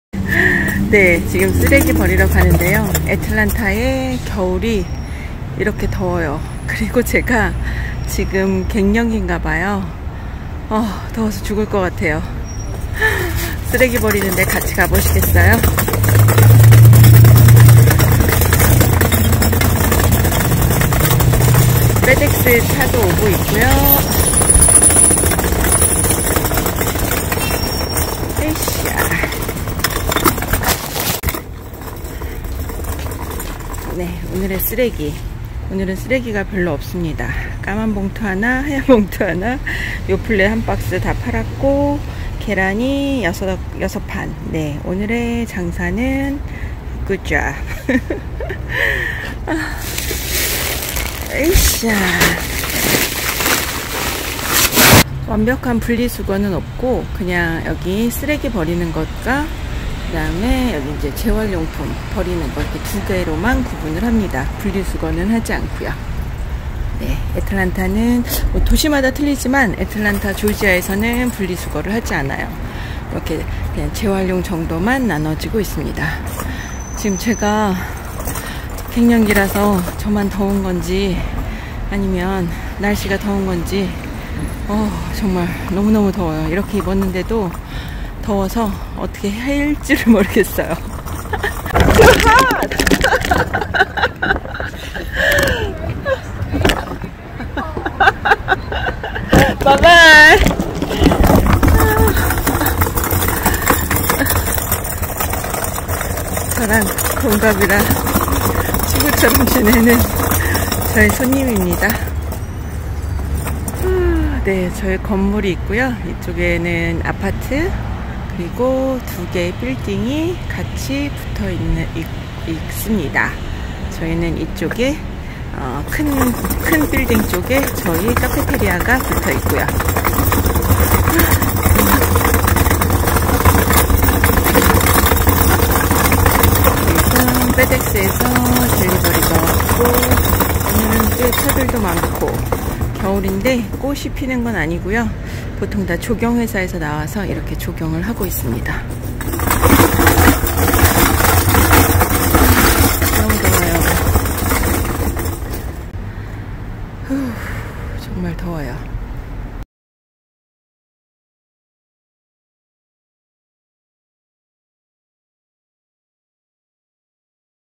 [미국일상] 50대 갱년기? 1월 겨울 불타오르네~~Fire
2020년 1월 15일 너무 더운날.
반팔 옷을 입고도 더워서 어쩔줄 몰라서 냉장고 문을 몇번씩 열었습니다.
갱년기가 너무 심한가 슬퍼하다가
날씨가 이상 기온이라는 말에 조금 위안을 삼으며 이날을 남겨볼려고요.